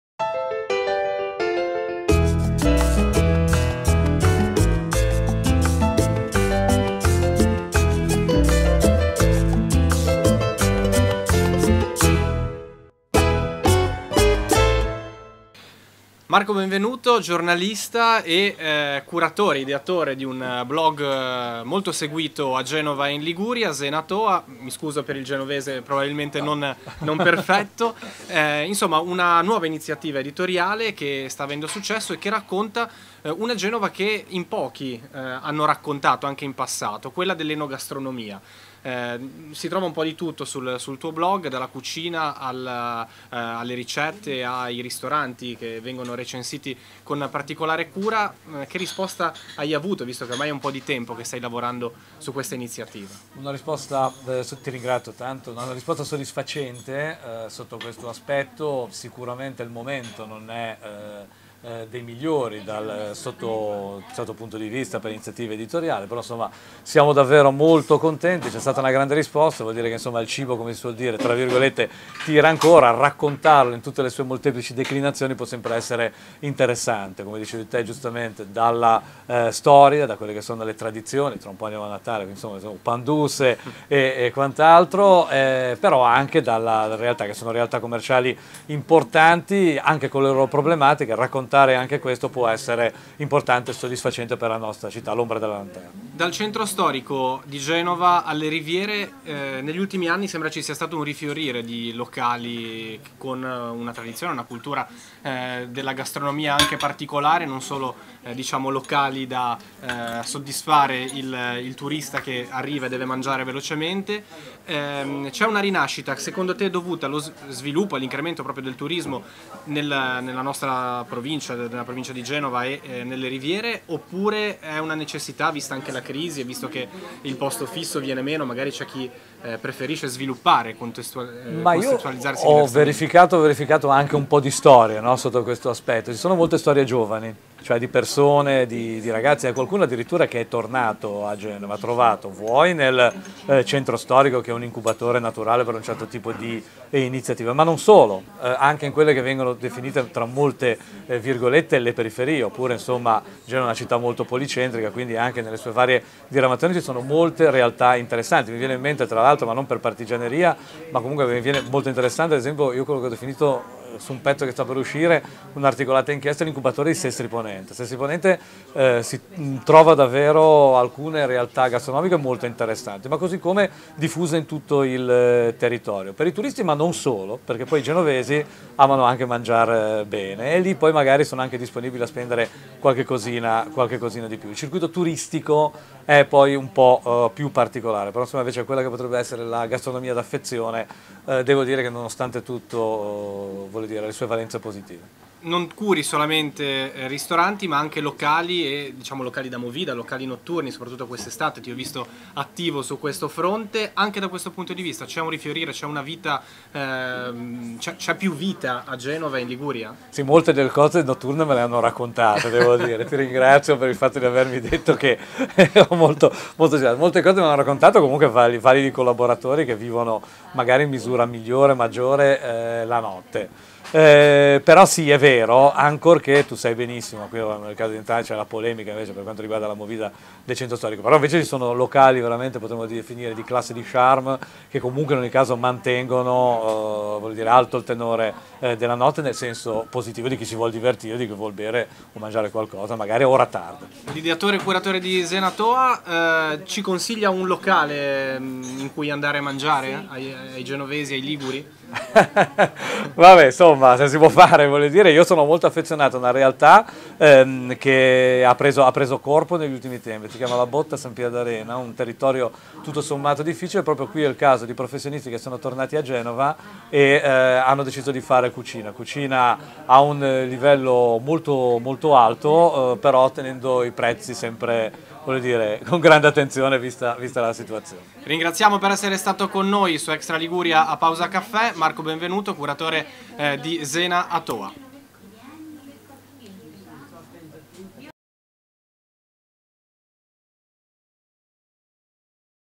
Così metto la Liguria a tavola
Il giornalista genovese Marco Benvenuto è protagonista a pausacaffè per parlare di enogastronomia a Genova e nelle riviere. Il suo blog "Zena a Toua" sta diventando un punto di riferimento per gli appassionati del genere.